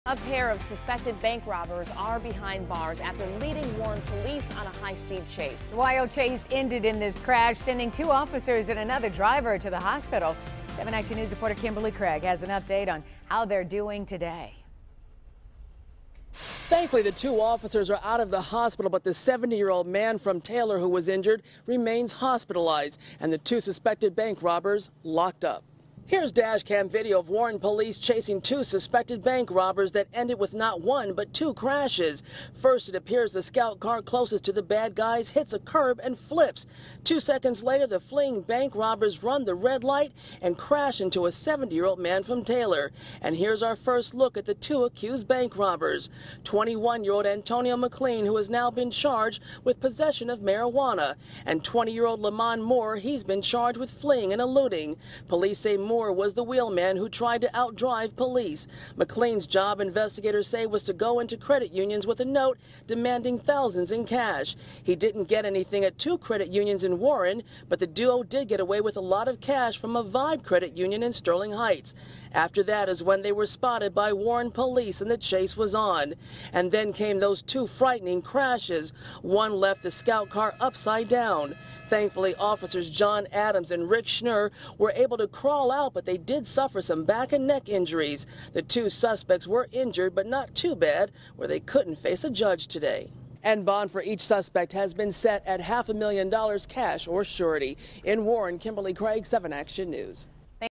Video shows Warren police SUV flip after chasing bank robbery suspects
New video given to 7 Action News shows a Warren police SUV flip while chasing two bank robbery suspects on Thursday afternoon.